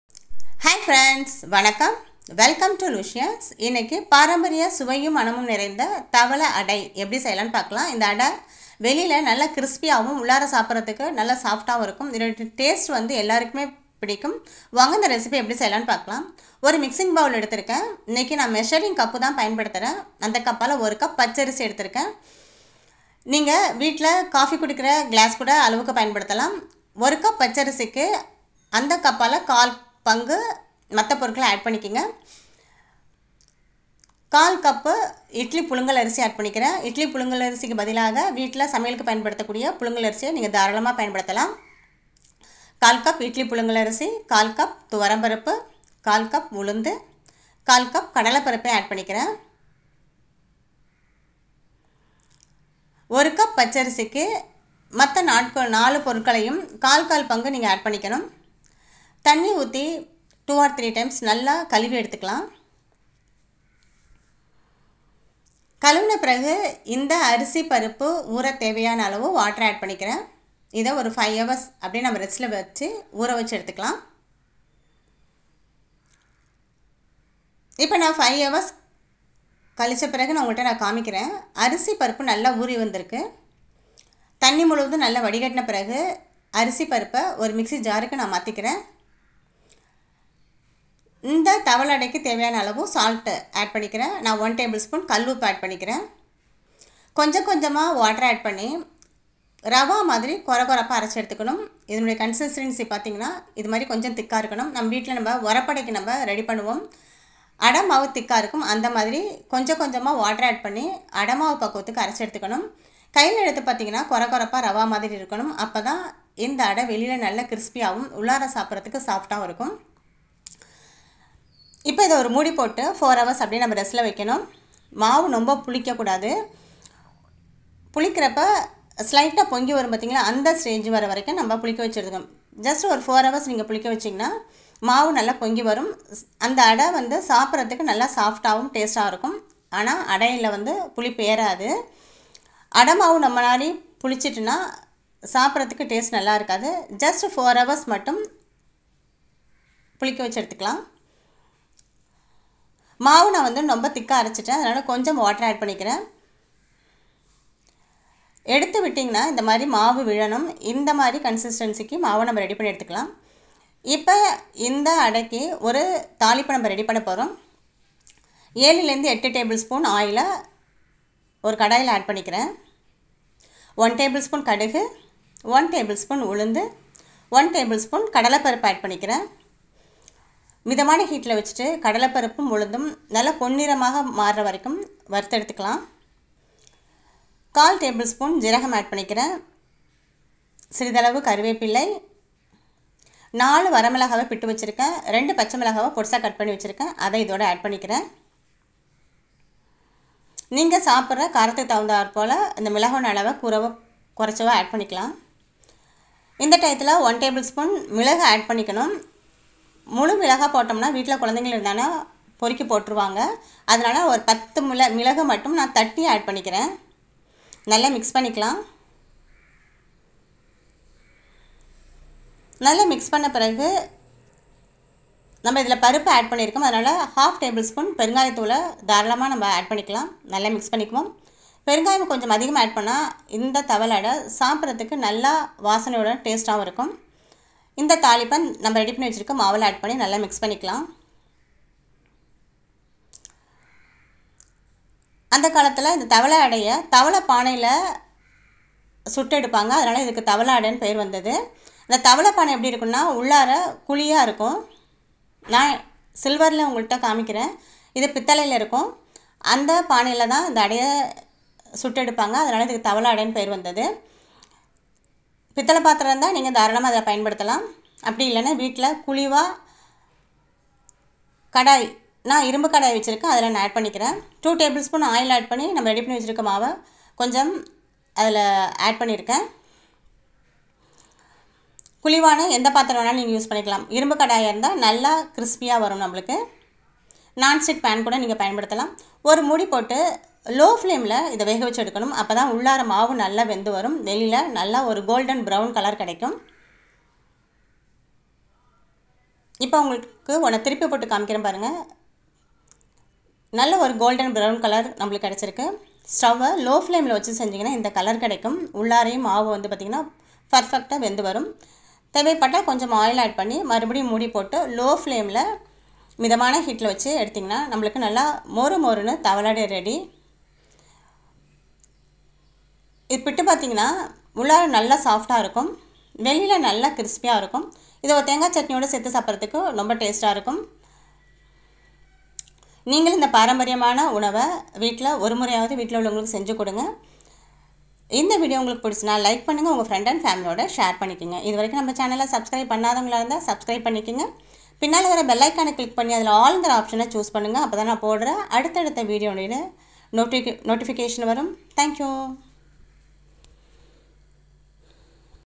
இட்லி ,தோசைக்கு பதிலாக பாரம்பரியமான மொறு மொறு கர கர தவல அடை |Thavala adai in tamil#LUSCIOUS5
பாரம்பரியமான   மொறு மொறு கர கர  தவல   அடை  |Thavala adai in tamil#LUSCIOUS5

INGREDIENTS
raw rice 1 cup
idly rice 1/4 cup
door dal 1/4 cup
urad dal 1/4 cup
bengal gram 1/4 cup
oil salt
bengal gram 1 tbsp.
peeper 10 no.
red chill
green chilli
hinge 1/2 tbsp.
curry leaves

jeera 1/ 4 tbsp.